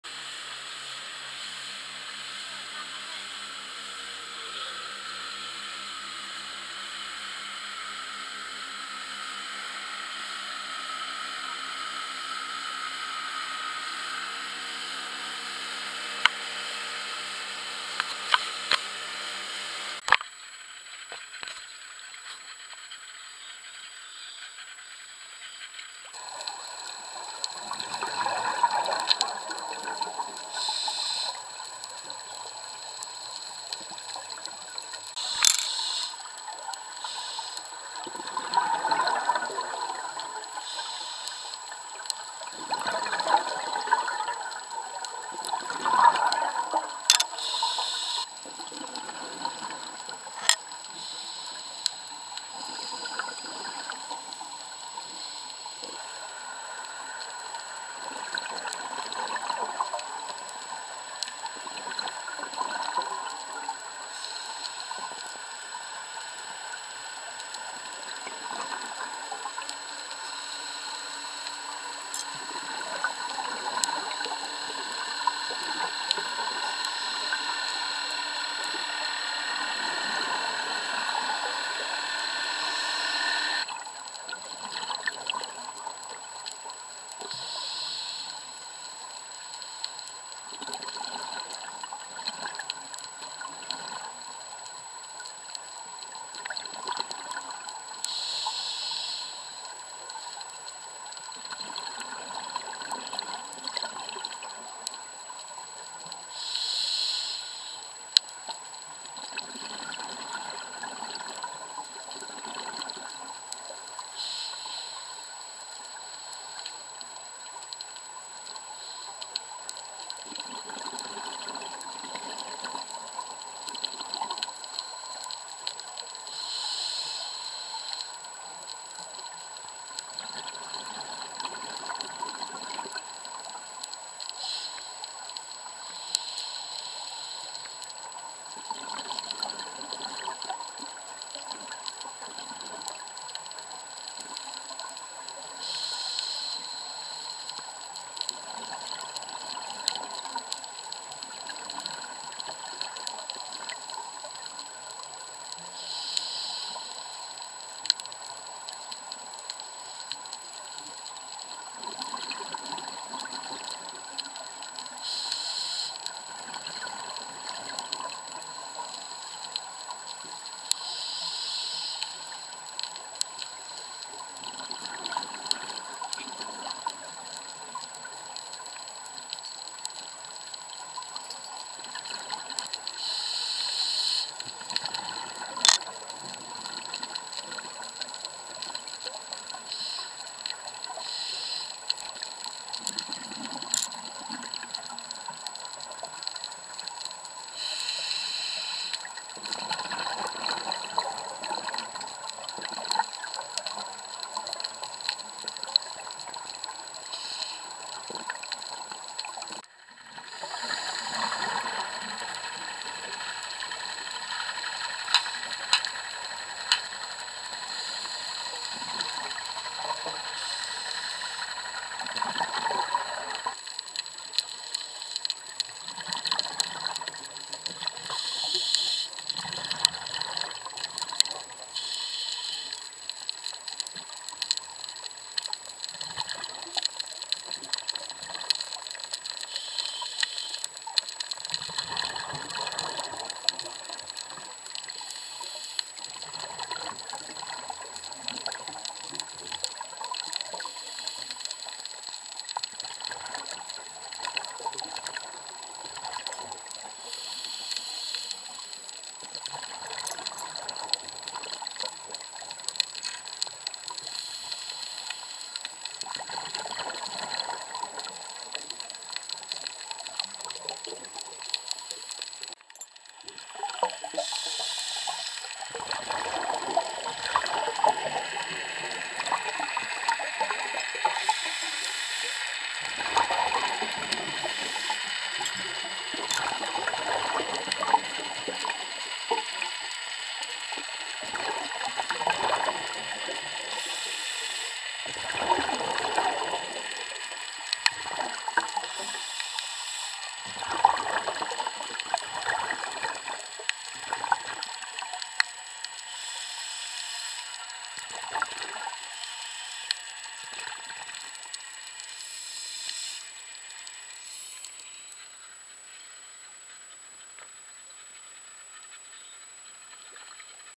Average dive at Nusa Penida, Bali
Good divers and very little current at dive site SD (sekola desa). Average day at Penida's amazing northern slope. Raw cut, no color correction. Recorded with an Intova action cam.